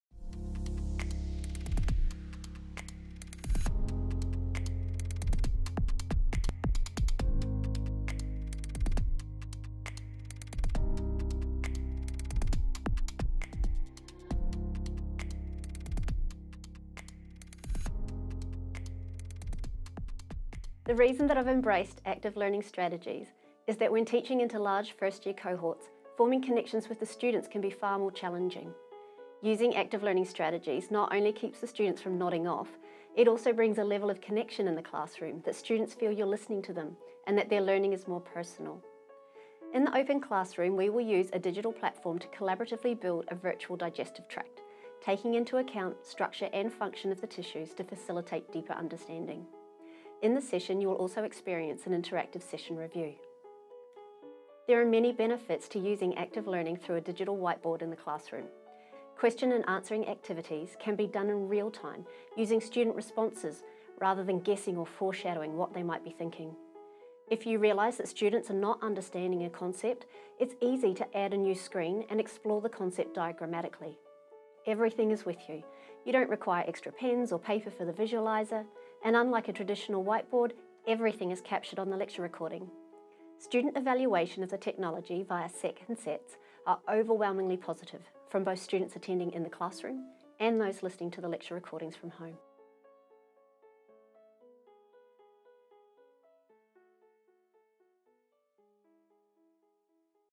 Open Classes - Charlene Willis - Utilising the digital whiteboard when lecturing
Charlene Willis outlines her contribution to Griffith's Celebrating Learning & Teaching Week 2019.

During her active learning session, participants will use the digital whiteboard to assemble the human digestive tract, from ingesting food, to excreting waste products.

This activity will enhance understanding of the biological process of getting food into, and traveling through, the digestive tract.

This exercise highlights  the benefits of using a digital whiteboard instead of traditional slides in lectures which contain large first year cohorts.